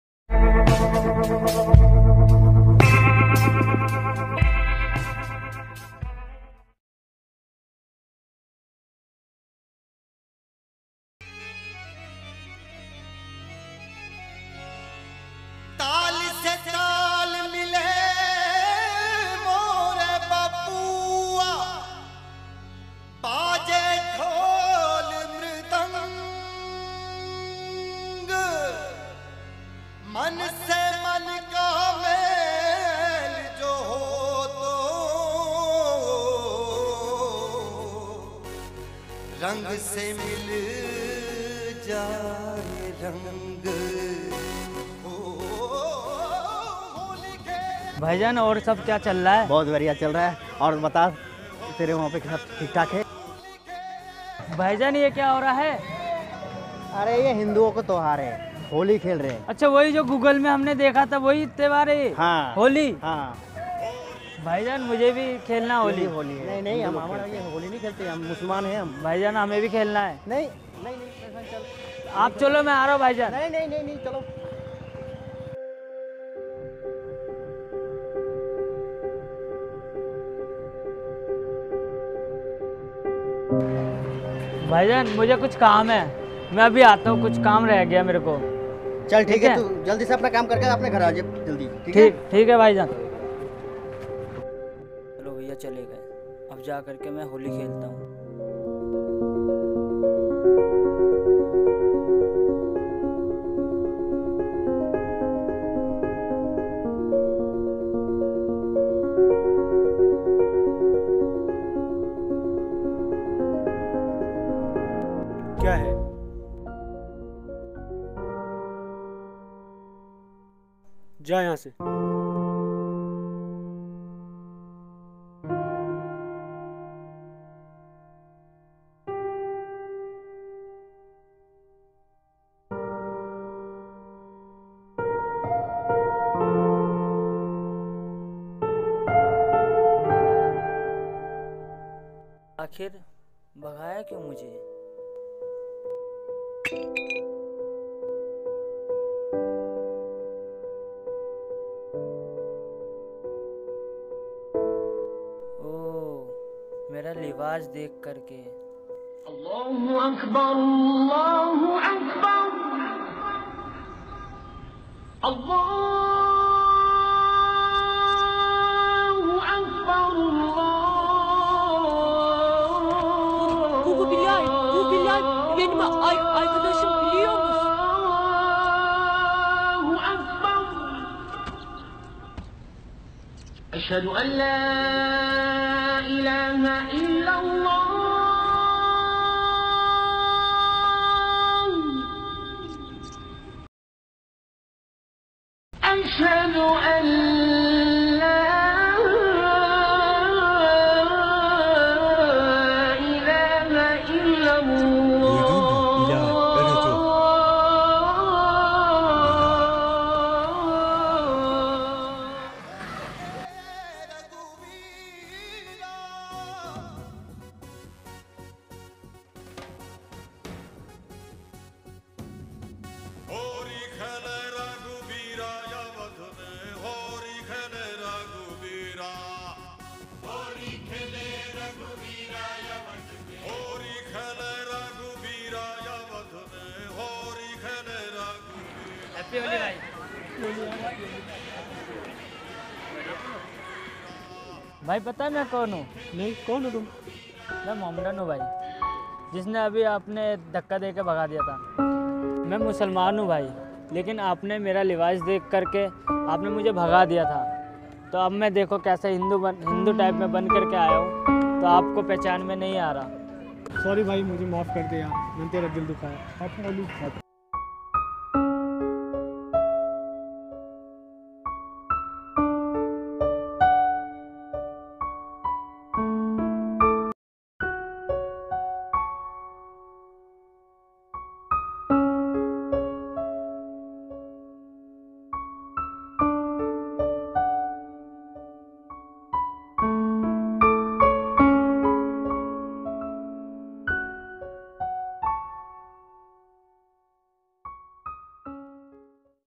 A Holi with Muslim - Halder Mind | Story Of Humanity | Holi Video | Happy Holi 2020
A Holi with Muslim - Halder Mind | Holi Video | Happy Holi 2020 | Based On Social Awareness & Story Of Humanity

Please Support Us Friends
Spread The Love & Show The Humanity

Team
Halder Mind Production

Video Directed by - Mithun Halder
Story Written By - Bappa Halder
Cast - Mohit Sharma, Bappa Halder, Mohan Kumar, Yashraj, Suraj Saxena, Kavya Sharma, Pooja, Animesh Das, Pihu Roy